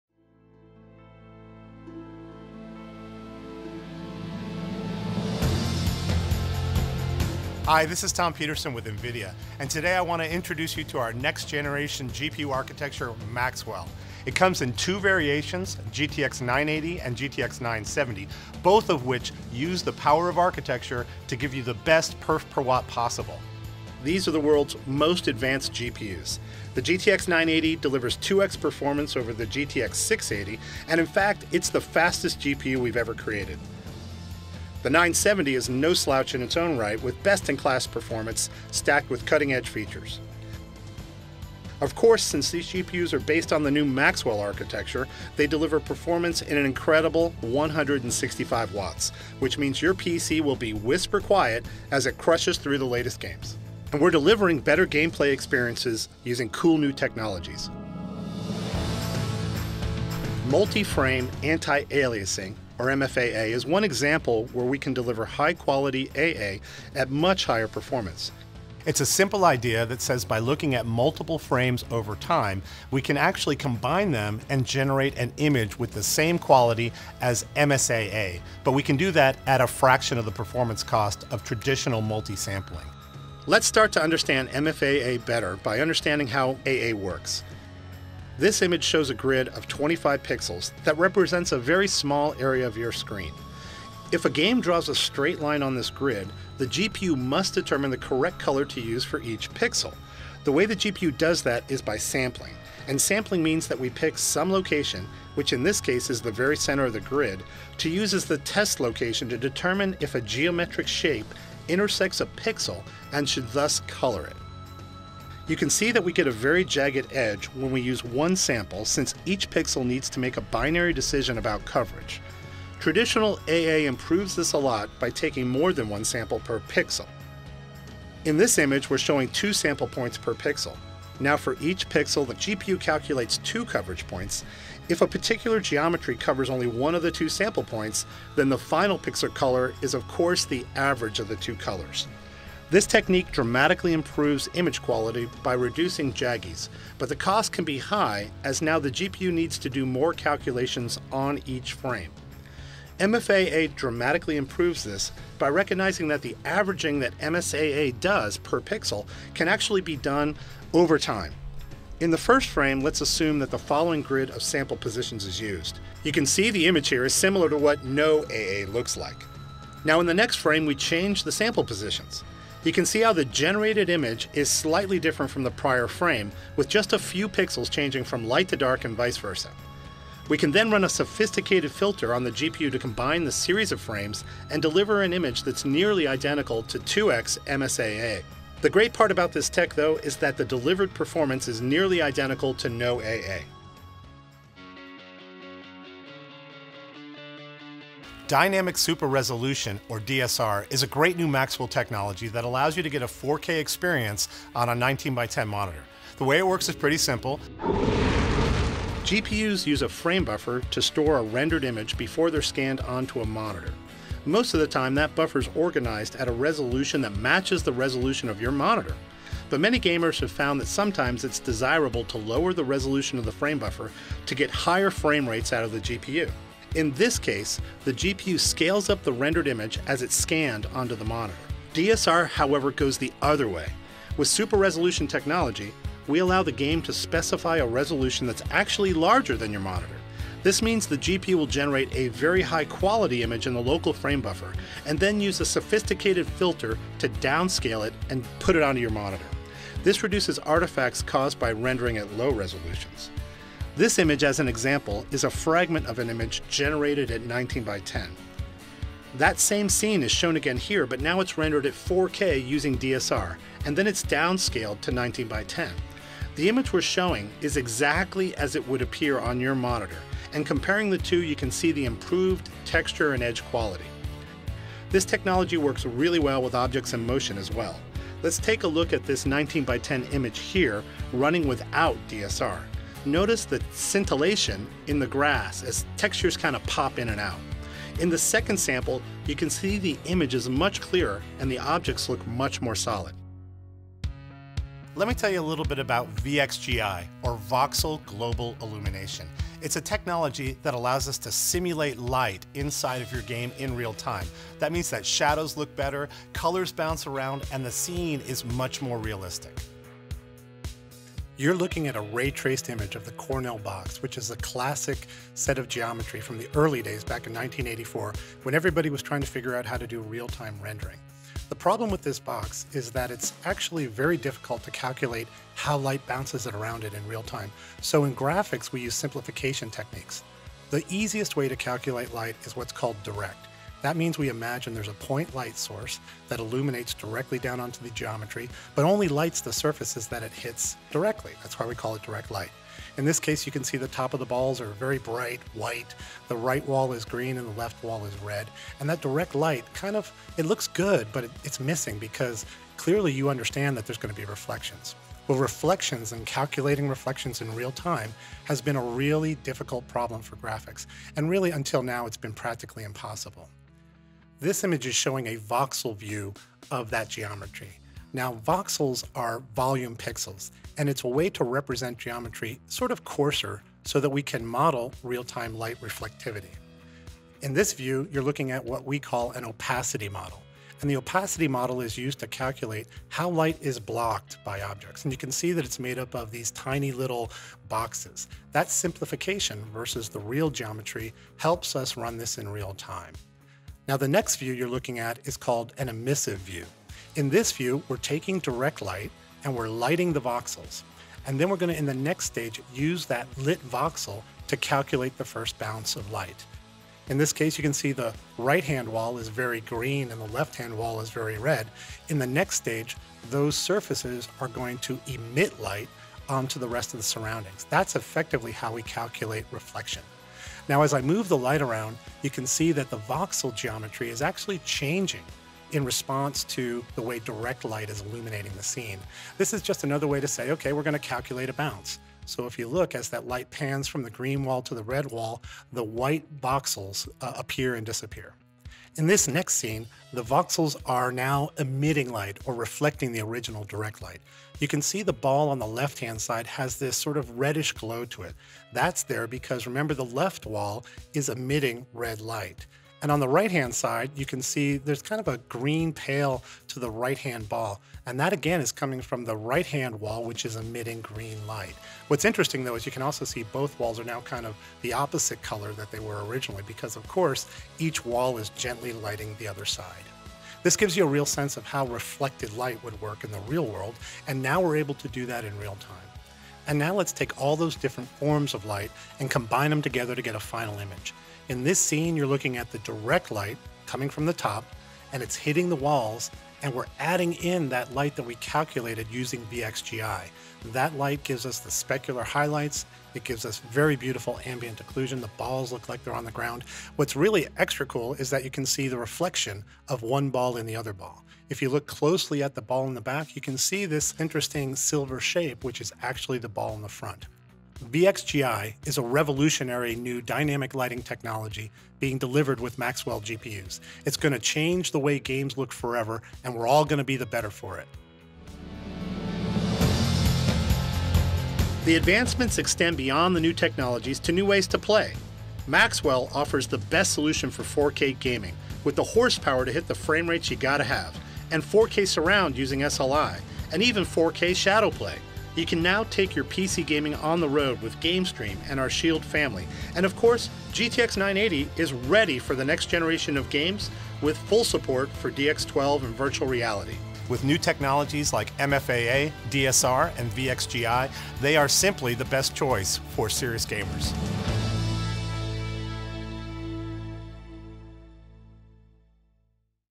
GeForce GTX 980 & 970 Product Video
Discover the GeForce GTX 980 and 970—the world’s most advanced graphics cards. Powered by NVIDIA Maxwell architecture and inspired by light, these GPUs include incredible performance and advanced technologies such as MFAA, DSR, and VXGI to deliver a truly elite gaming experience.

Game Advanced.
GeForce GTX 980 and 970.
Powered by Maxwell.